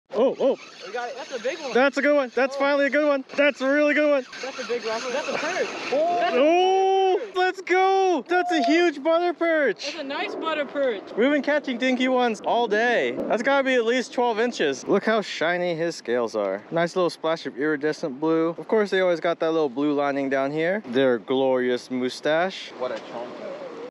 BEAUTIFUL Butter Perch :D! Coast Guard Pier, Monterey
In this video, I catch a beautiful, chonky 12-inch or more Butter Perch at Coast Guard Pier! :D @PierLifeFisherman and his friends join me for this session, for the day we caught (kept and released) a total of 60 fish! :O Crazy day of fishing, mainly a lot of dinky Black or Blue Rockfish, Blacksmith Perch, a few Painted Greenlings, and it started to rain too haha

Main Video in case you're interested, it's a long one, bear with me xP: